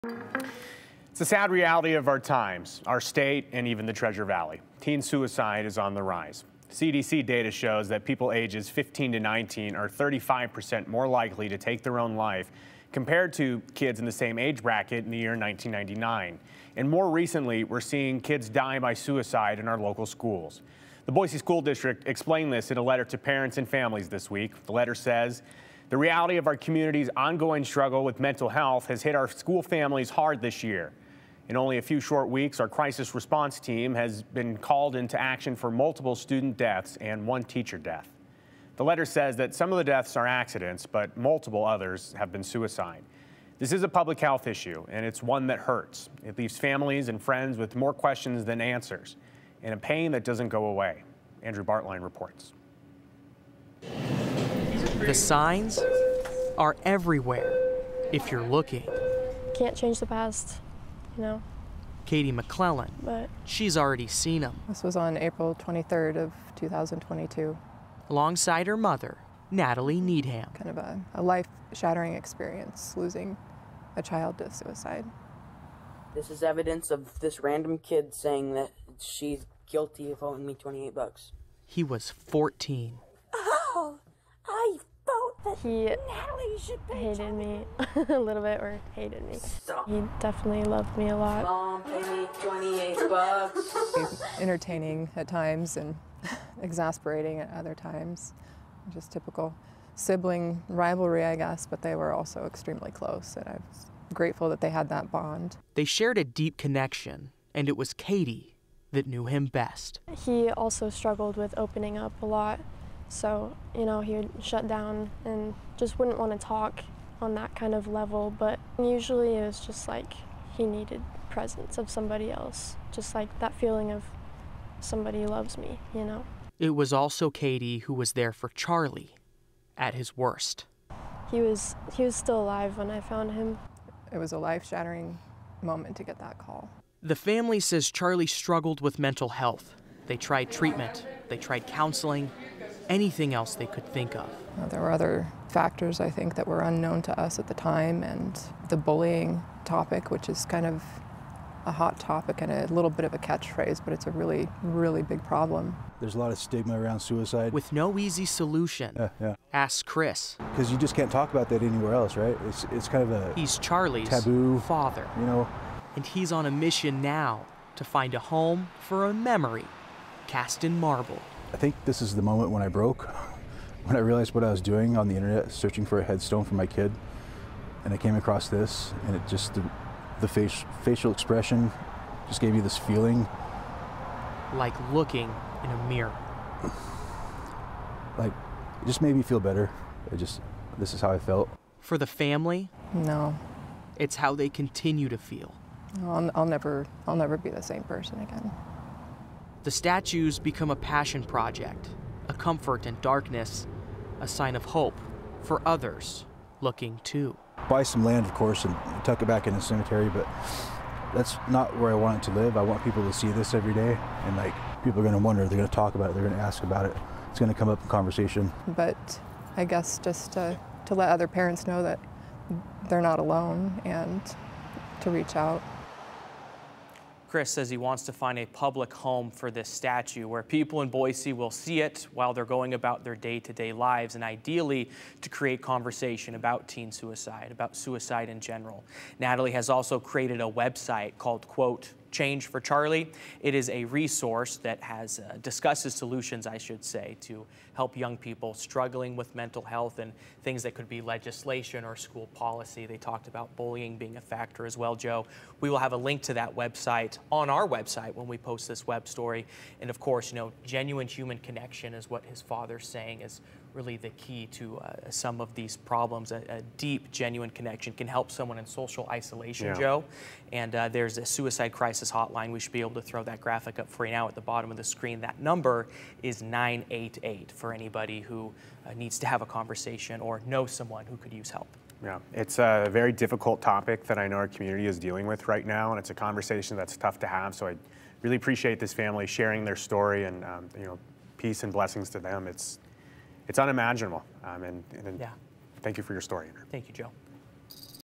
'Change for Charlie': Boise family spreads awareness about teen suicide
The family of Charlie wants to find a public space for the statue and hopes parents come together to speak about teen suicide.